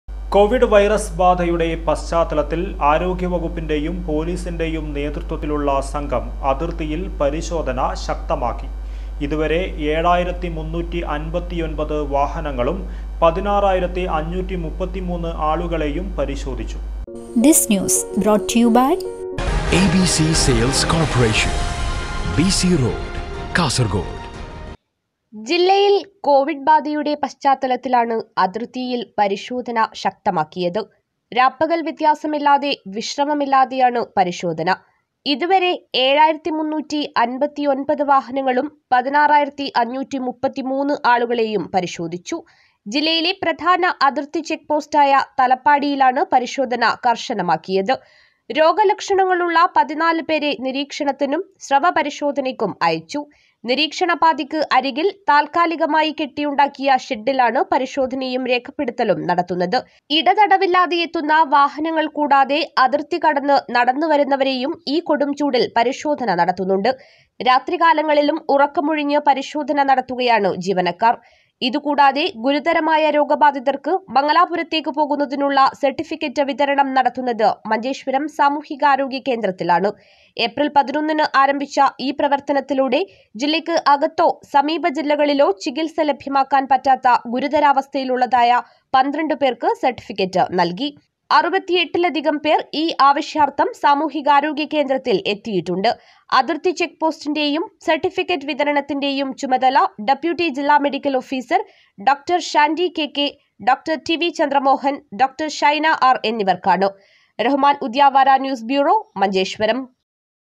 അതിര്‍ത്തിയില്‍ പരിശോധന ശക്തമാക്കി.
കോവിഡ് ബാധയുടെ പശ്ചാതലത്തില്‍ ആരോഗ്യവകുപ്പിന്റെയും പോലീസിന്റെയും നേതൃത്വത്തിലുള്ള സംഘം അതിര്‍ത്തിയില്‍ പരിശോധന ശക്തമാക്കി. ഇതുവരെ 7359 വാഹനങ്ങളും 16533 ആളുകളയും പരിശോധിച്ചു.